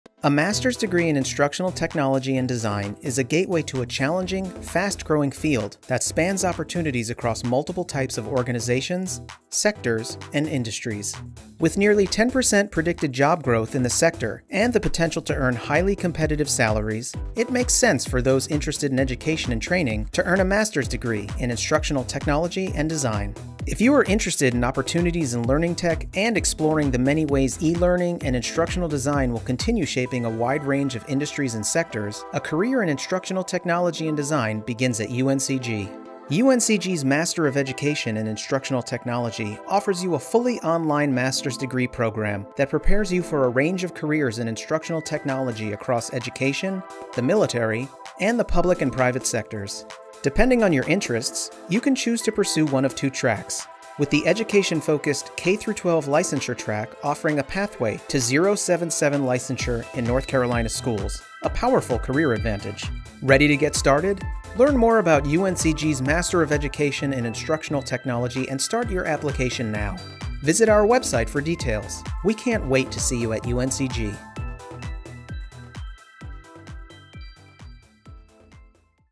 M.Ed in Instructional Technology & Design at UNCG
Students in UNC Greensboro’s Master of Education in Instructional Technology gain hands-on experience in the design, development, implementation, and evaluation of educational media and technology.

Earning a master’s degree in instructional technology is the gateway to a fast-growing career field with opportunities across different sectors including higher education, nonprofit organizations, government agencies, healthcare, and more.